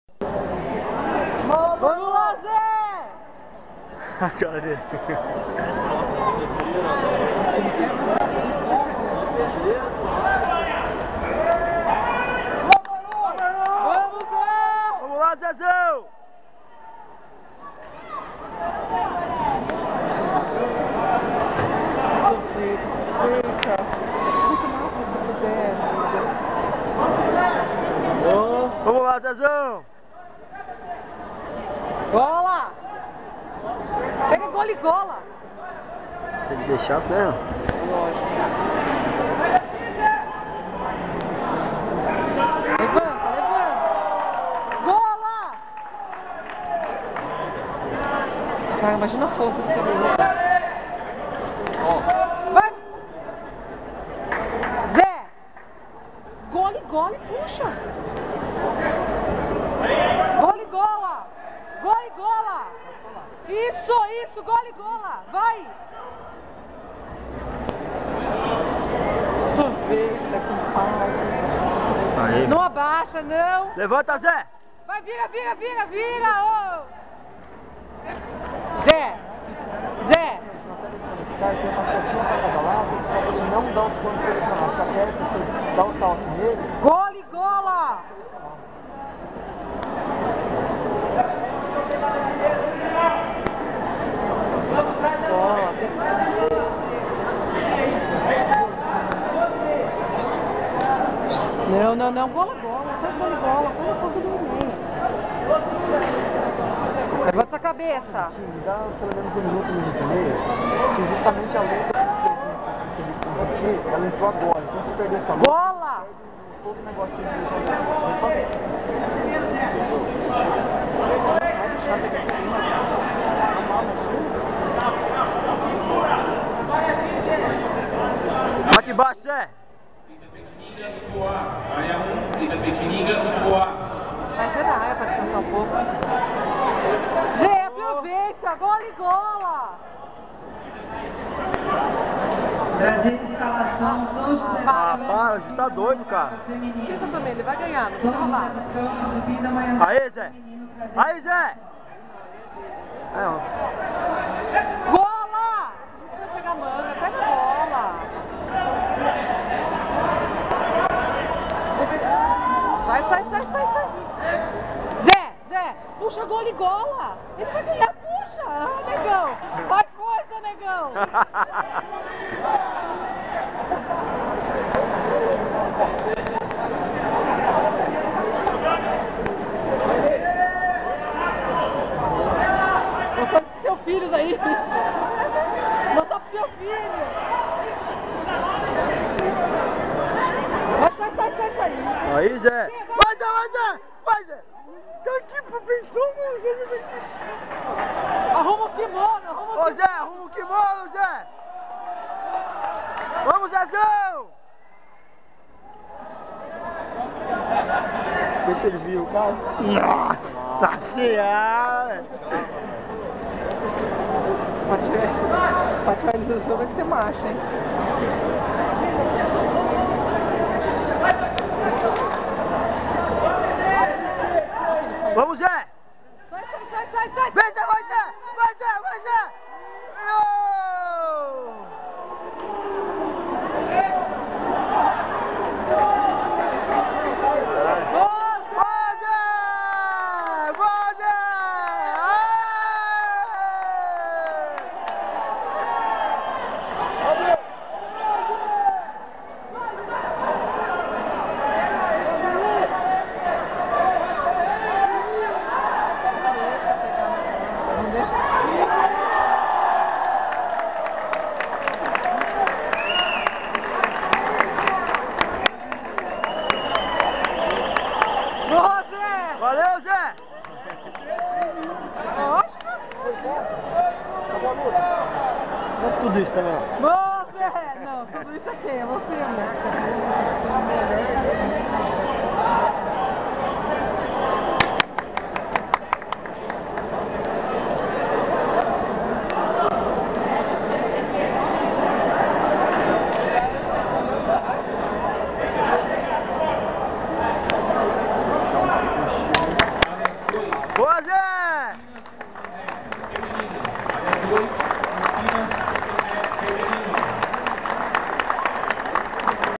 SUNP0003
Ilias Iliadis  vs  Josecarlos Freire , abertos 2012  Mogi x Araçatuba